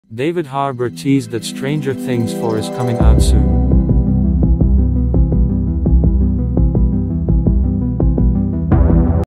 Stranger Things 4 - David Harbour Teases Release Date For SEASON 4
In this short episode I'll talk about how last night at the New York Kincks game David Harbor and Noah Schnapp were captured live, David showed his phone where it said, "ST4 COMING SOOON, PROMISE"